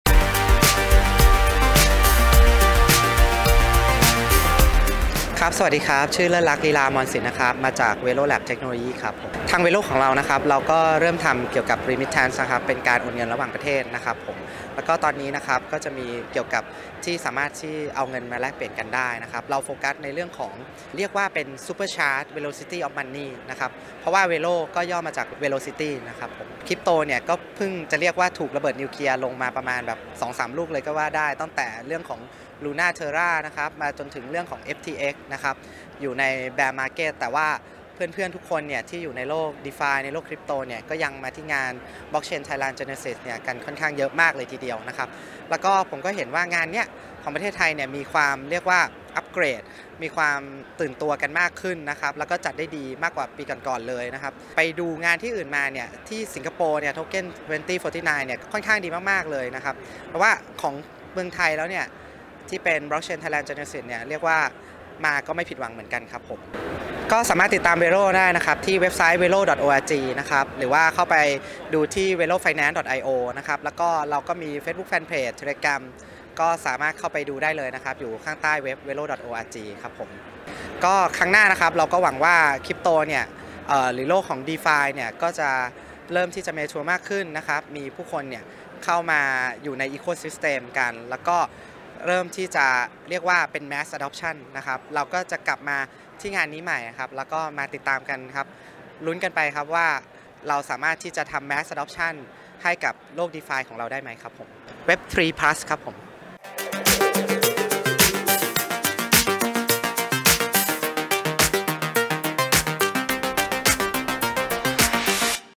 Blockchain Thailand Genesis 2022 | Interview คุณเลิศลักษณ์ ลีลาอมรสิน จาก Velo Labs Technology
สิ้นสุดไปแล้วกับงานบล็อกเชนที่ใหญ่ที่สุดในประเทศไทยซึ่งจัดขึ้นเป็นปีที่ 5 และยิ่งใหญ่กว่าทุกปี “Blockchain Thailand Genesis 2022” โดยตลอดวันงานทั้ง 2 วันนั้น เราได้ยกผู้มีประสบการณ์และวิทยากรผู้เชี่ยวชาญในอุตสาหกรรมบล็อคเชนและสินทรัพย์ดิจิทัลระดับโลกกว่า 100 ท่าน
มาร่วมพูดคุยและให้ความรู้กับคนไทย
📌วันนี้เรานำ Exclusive Interview กับ คุณเลิศลักษณ์ ลีลาอมรสิน Head of Commercial & Partnership จาก Velo Labs Technology ซึ่งเป็นหนึ่งใน Platinum Sponsor ของงานเราในปีนี้ มาให้เพื่อนๆ ฟังกัน